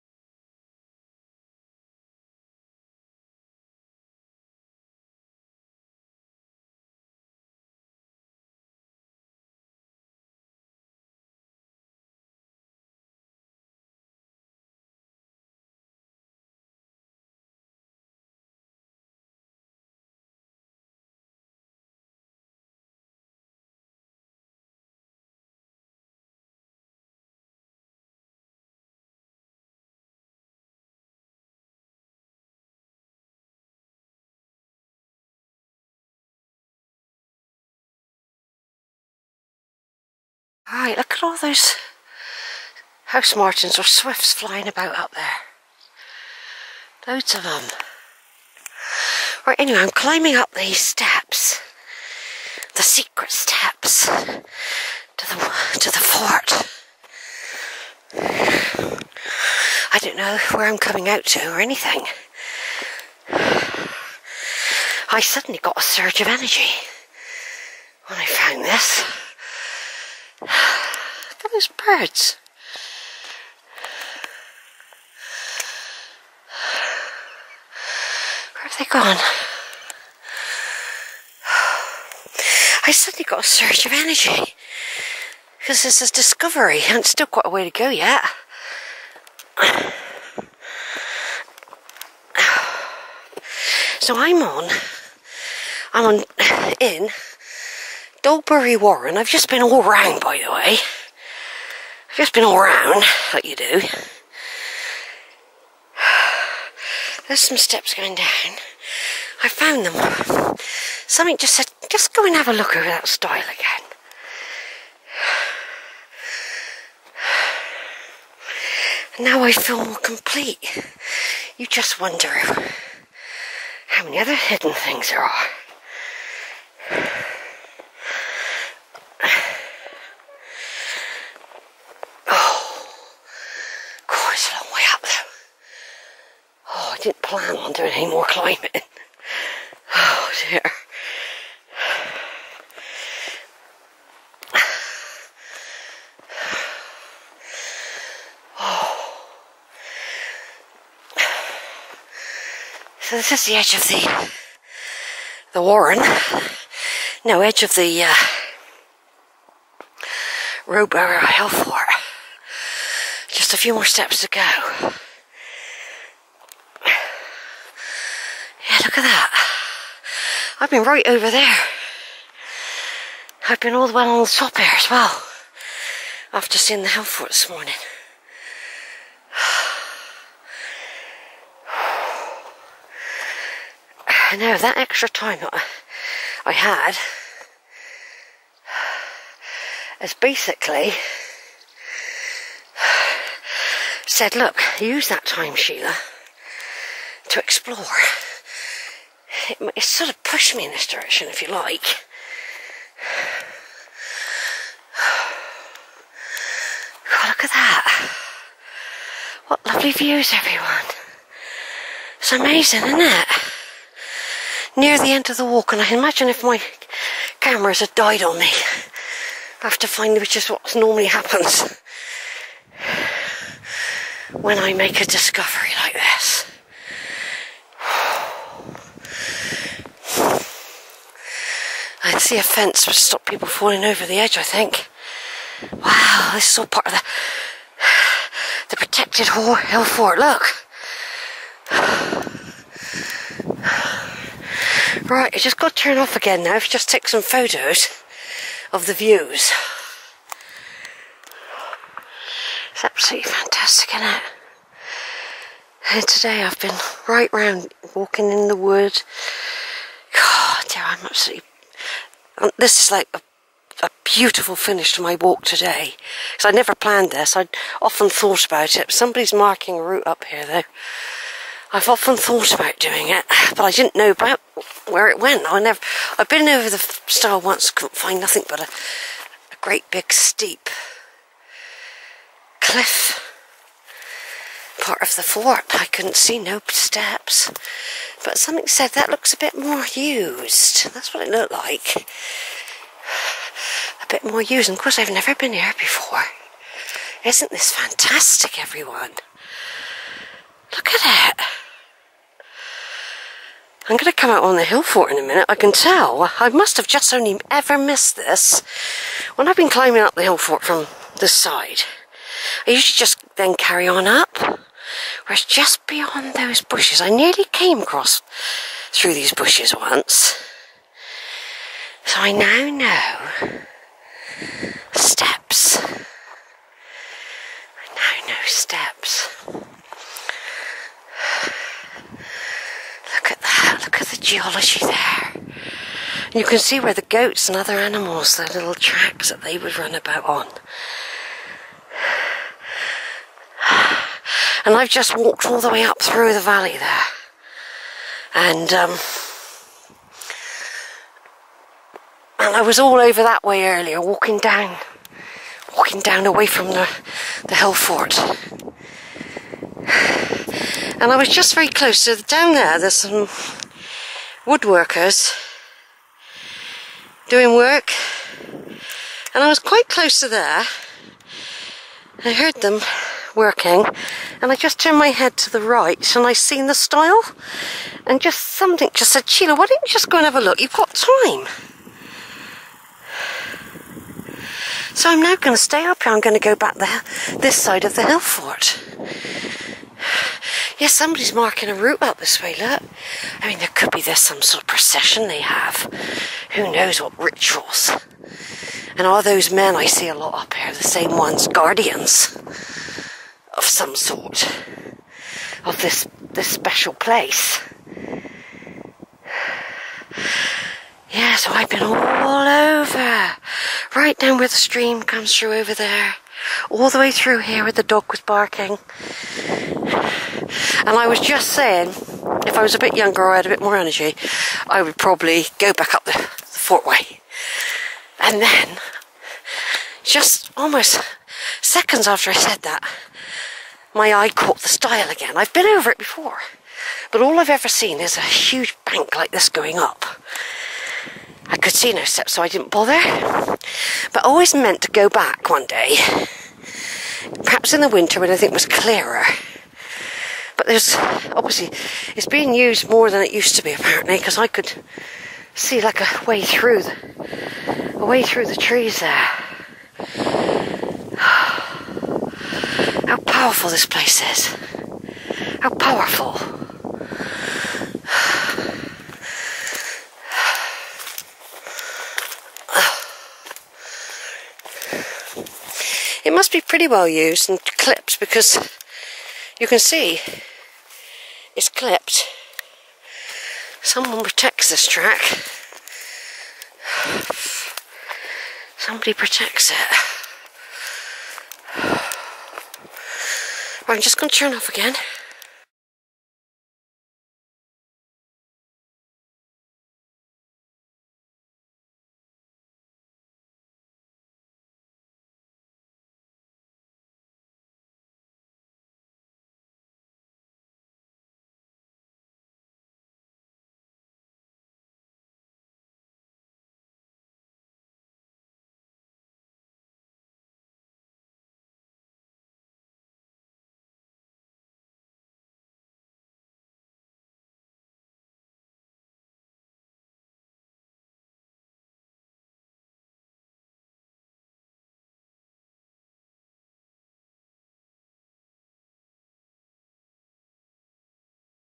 Rowberrow Hillfort & Warren, Part XII, 'Up The Steep Secret Steps of the Hillfort on Dolebury Warren
This is a really joyous moment finding these steps, another way up to the fort, other amazing, magnificent, spectacular views AND a wee bit dangerous as well--narrow, steep steps on the side of a steep rampart of a hillfort--a special entrance---I feel like I have just entered the 'Secret Garden' or 'the rabbit hole of 'Alice In Wonderland'--such is my joy---it is great making discoveries especially when one has been visiting a place for several years and there are still surprises, beautiful surprises and probably more in the future---This Hillfort has allowed me some access and has opened up a small window of its many treasures buried here---Greeeeeeeeeeeeeeeeeeeeeeeeeeeeeeeeeeeeeeat !!!